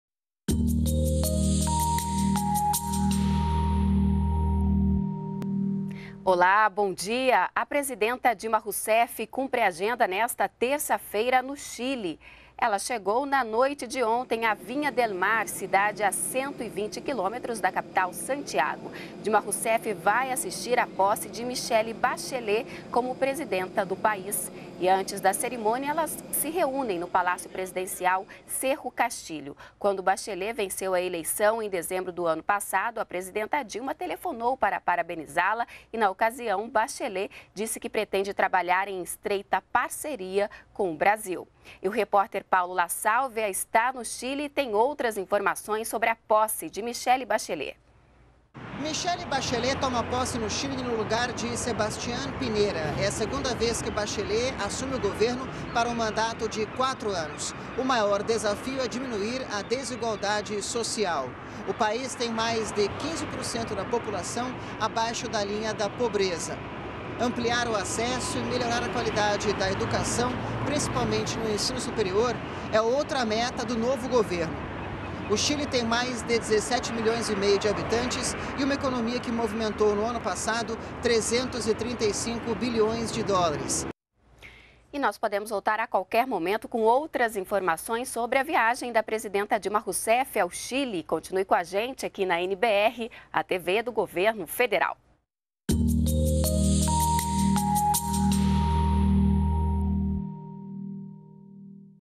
Dilma Rousseff está no Chile para posse presidencial de Michelle Bachelet
ATIVIDADES DA PRESIDENTA - 11.03.14: A presidenta Dilma Rousseff participa, nesta terça-feira (11), da posse de Michelle Bachelet como presidenta do Chile. Antes das cerimônias, Dilma e Michelle se reúnem no Palácio Presidencial de Cerro Castillo para tratar de projetos em conjunto. Esta é a segunda vez que Michelle é eleita para presidência do Chile. O maior desafio de Bachelet será diminuir a desigualdade social, já que 15% da população chilena está abaixo da linha da pobreza.